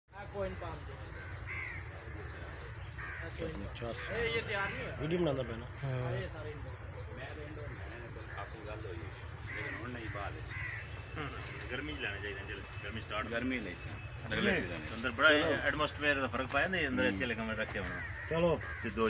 Pattoki nursery...Qasur Punjab
It's very impressive 👍 place...it's Pakistan biggest Nursery. In district Qasur Tehsil Pattoki near By pass Pattoki... Really enjoyed with friends ♥️